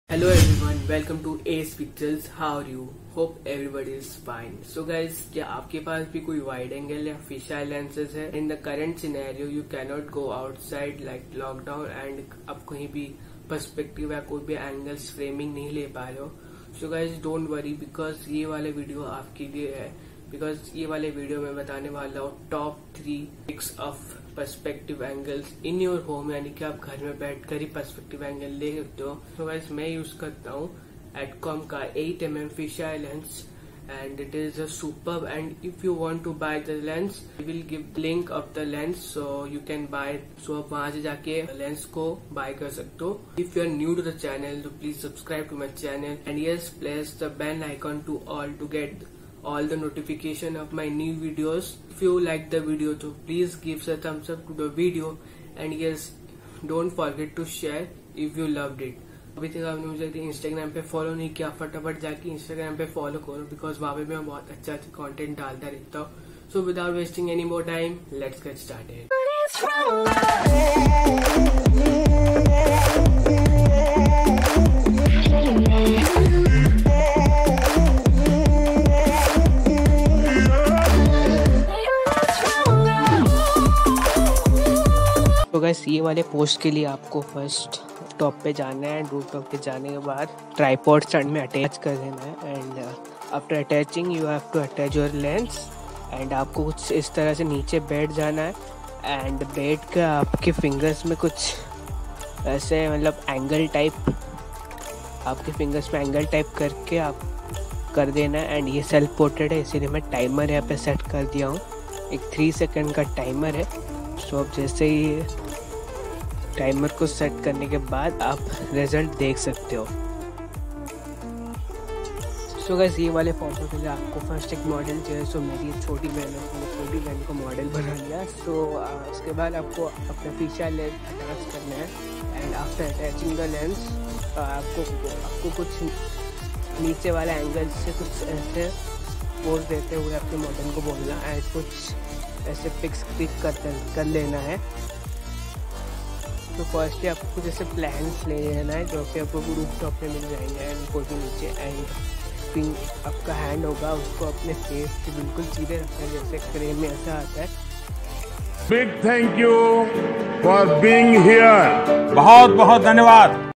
TOP 3 Perspective photography ⚡| Using wide angle and fisheye lens | AS Pictures
Hey guy's,
How are you all. Hope everyone is fine. In this video I have shown you TOP 3 perspective photography tricks in your house😉⚡.